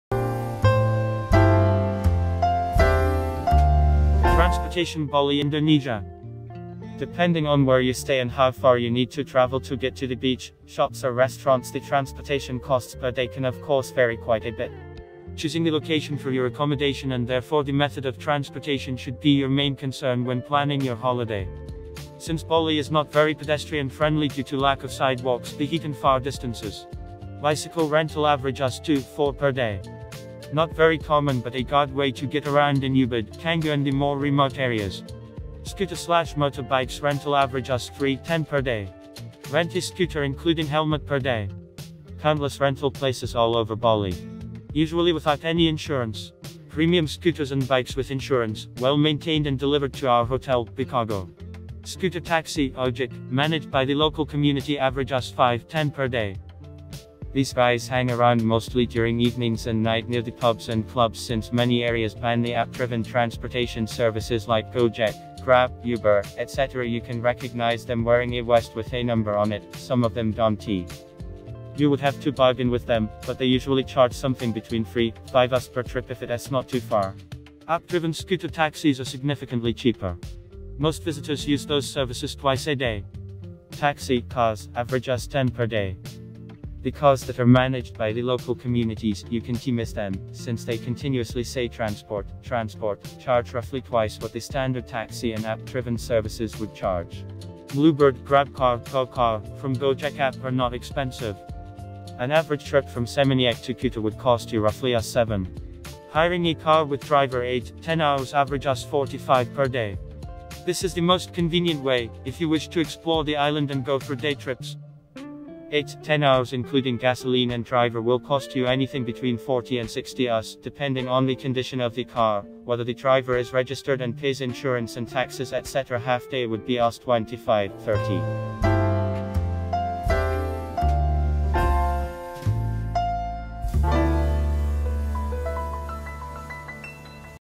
Transportation Bali Indonesia
Transportation Bali Indonesia. Depending on where you stay and how far you need to travel to get to the beach, shops or restaurants the transportation costs per day can of course vary quite a bit.